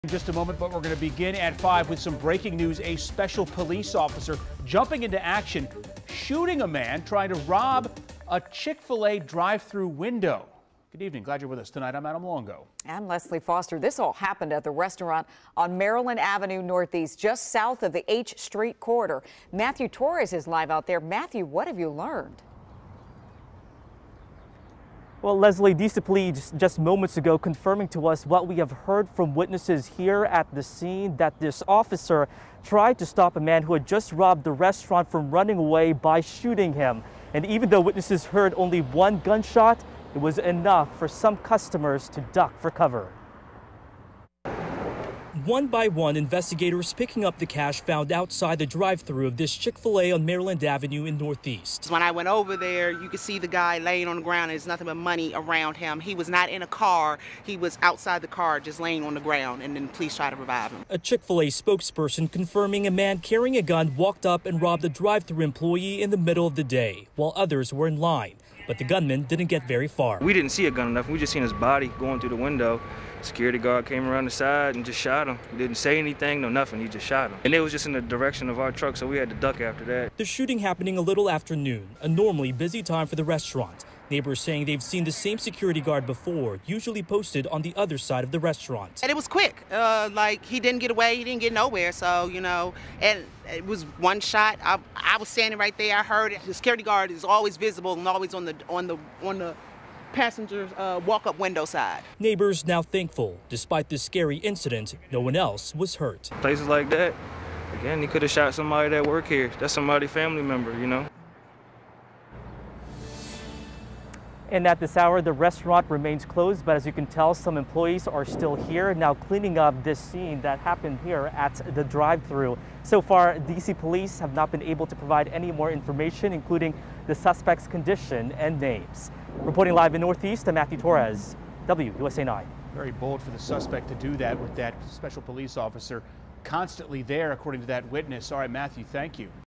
Man shot outside Chick-fil-A in Northeast, DC
DC Police said the victim is unconscious and unresponsive, but this has not yet been declared a homicide.